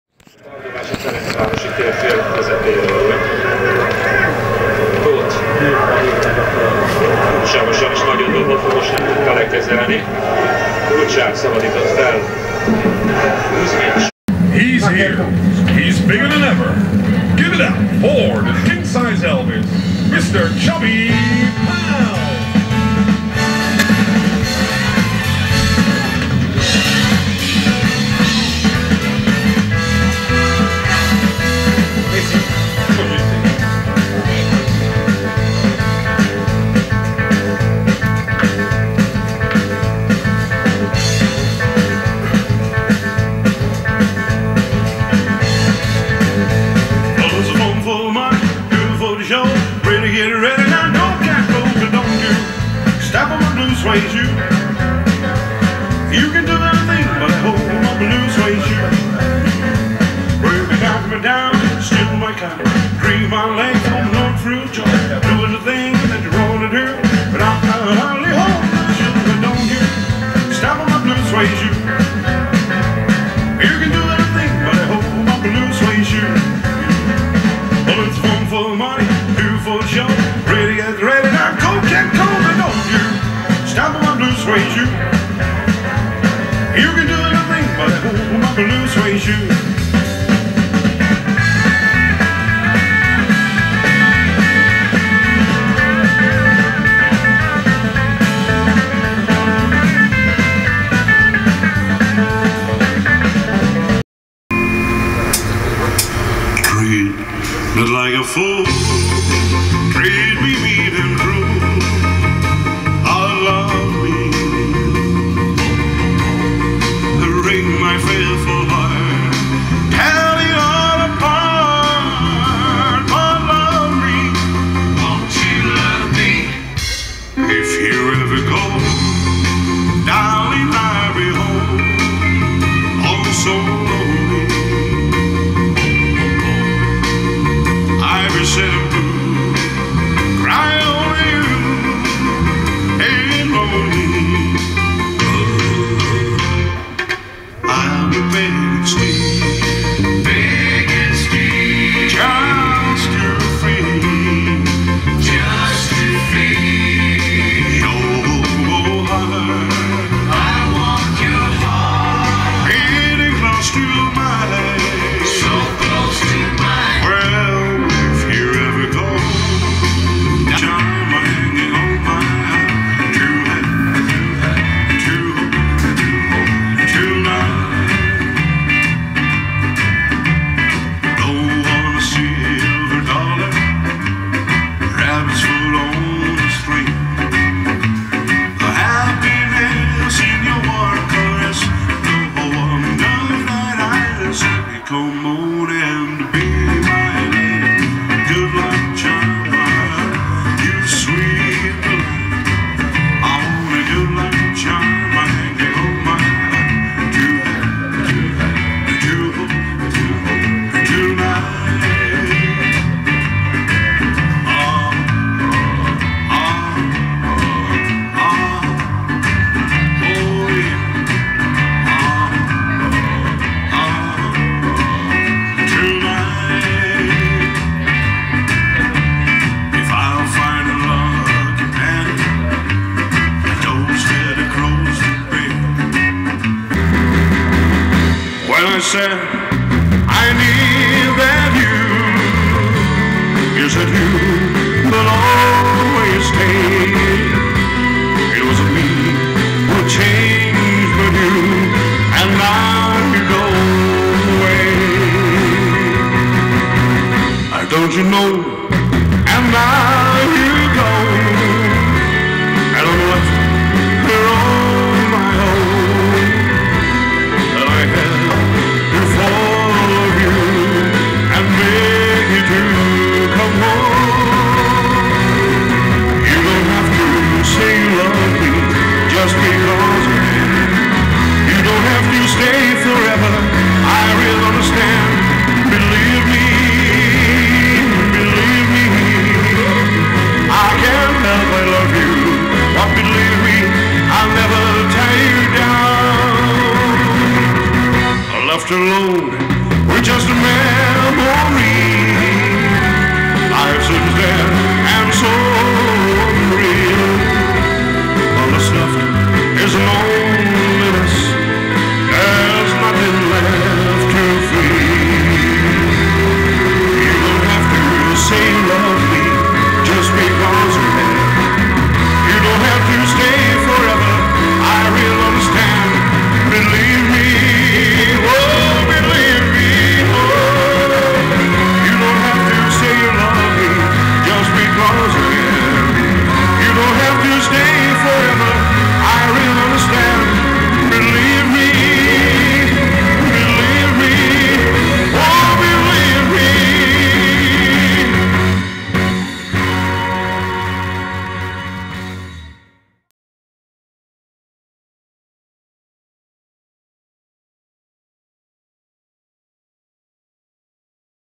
ANOTHER ELVIS CLONE
The guy who sings almost every Elvis's song. Met him in Zalakaros, Hungary